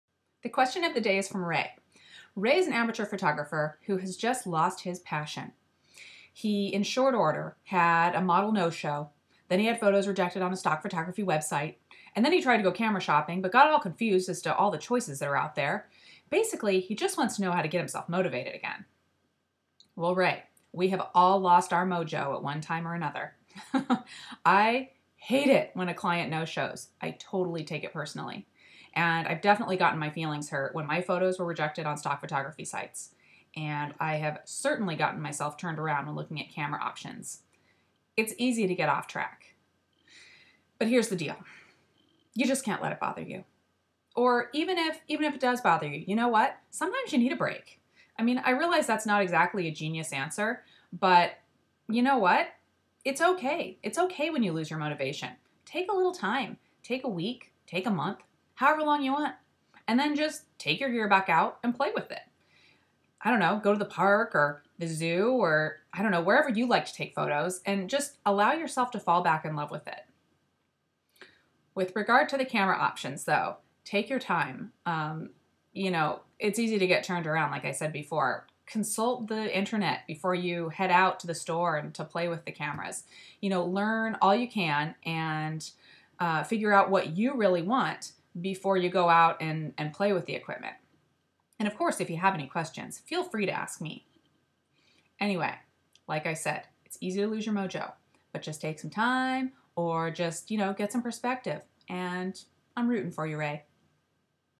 Motivation - The SnapChick Question of the Day
SnapChick answers your questions. In this video, she talks about losing your motivation!